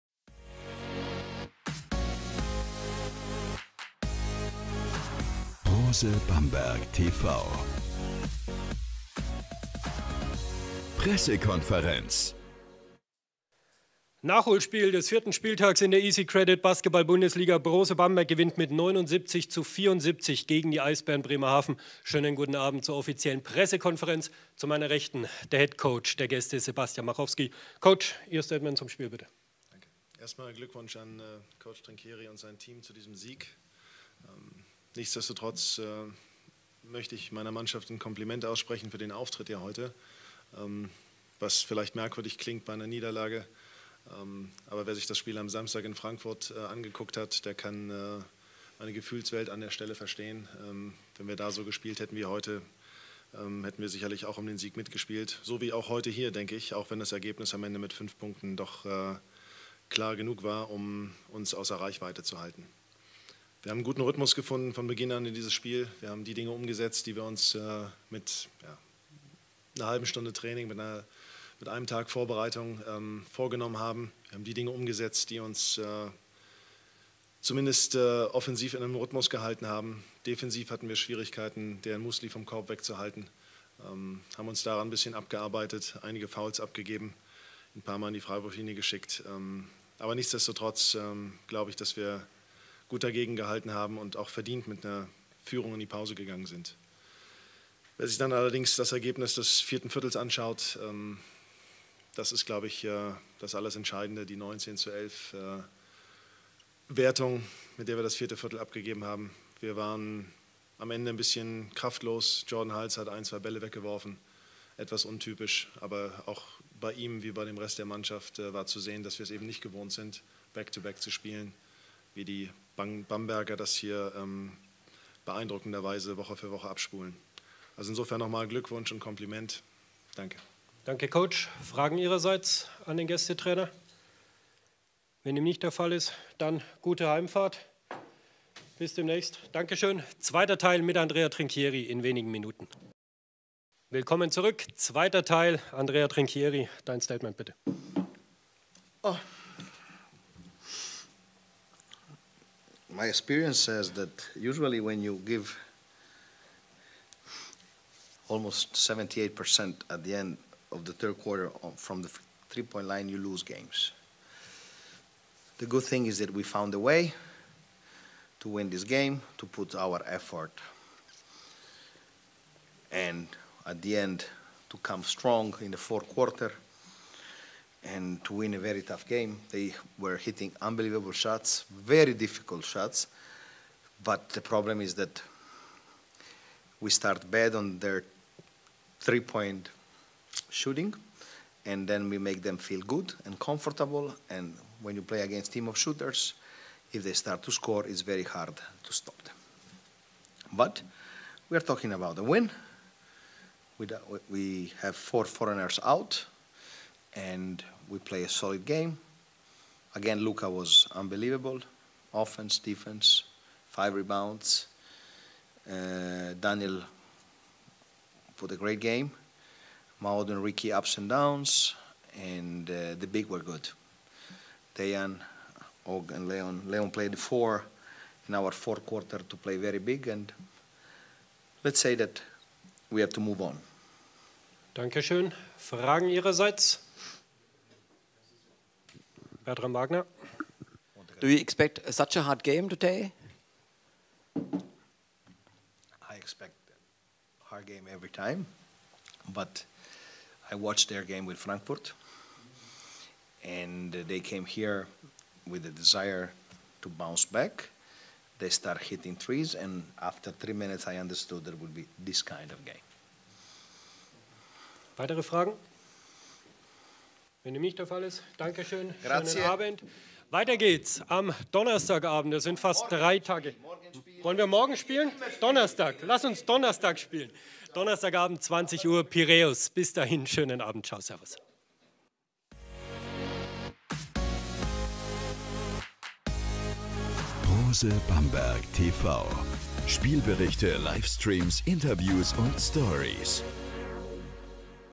Pressekonferenz Brose Bamberg vs. Eisbären Bremerhaven 79:74
Nachholspiel des 4. Spieltags: Sebastian Machowski und Andrea Trinchieri mit den Stimmen zum Spiel.